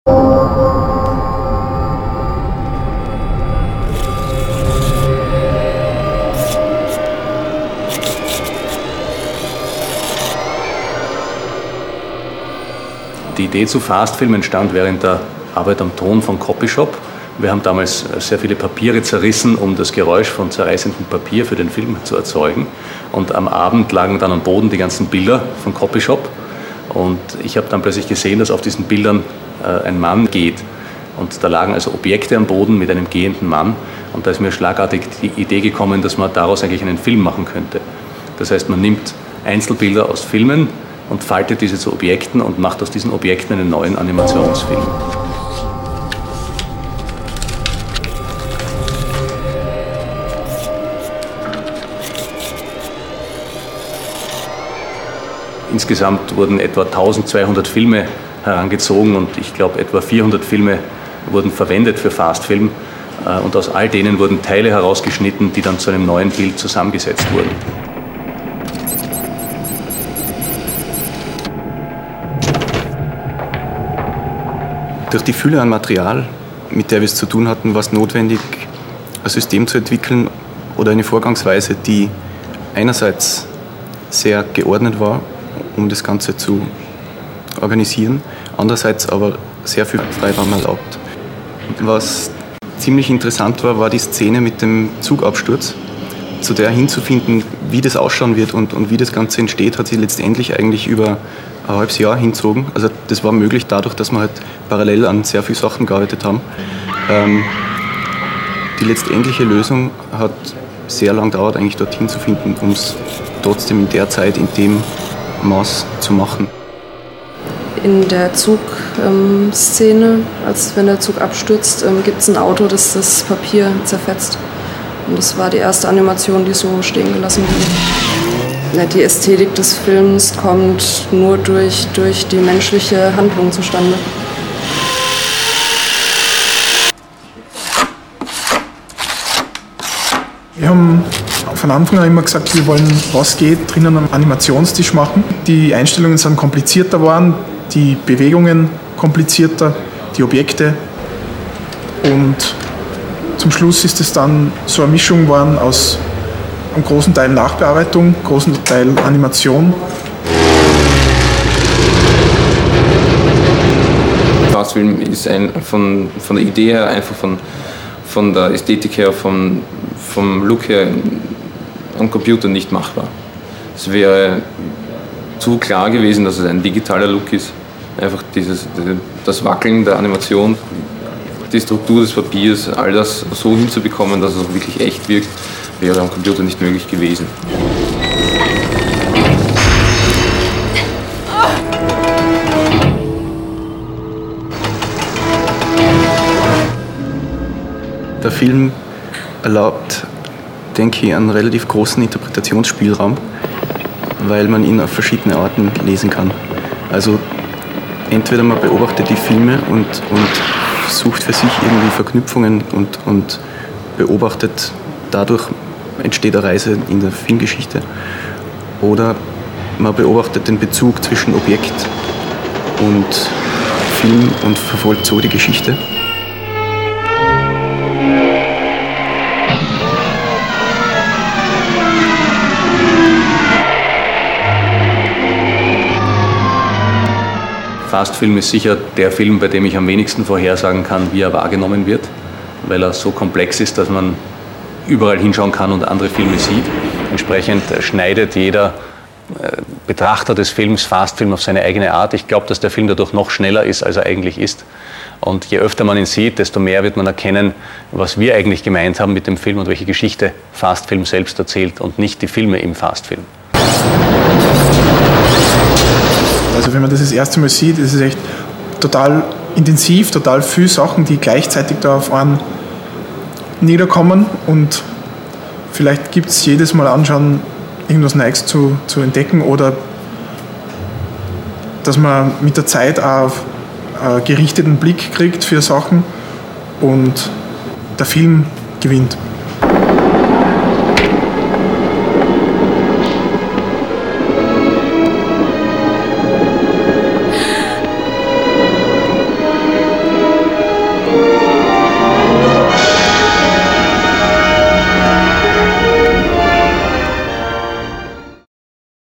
Making of Virgil Widrich's "Fast Film"
The Making of Virgil Widrich's "Fast Film". In 14 minutes, "Fast Film" provides a tour de force through film history, from its silent beginnings to present-day Hollywood. The filmmakers printed out some 65,000 individual images from 300 films, folded them into paper objects, arranged them in complex tableaux, and then brought them to life with an animation camera. Winner of 36 international awards.